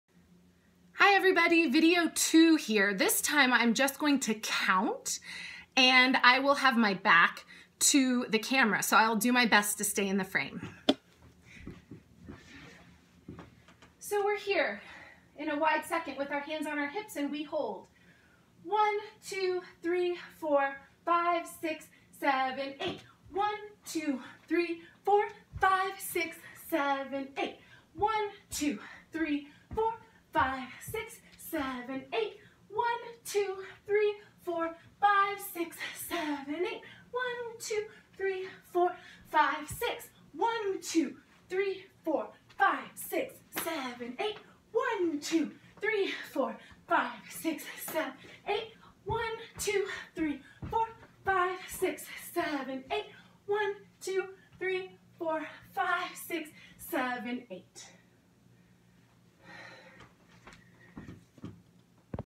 WWCF Audition Choreography
TSS audition workshop choreography. Video 2.
Back facing breakdown with counts.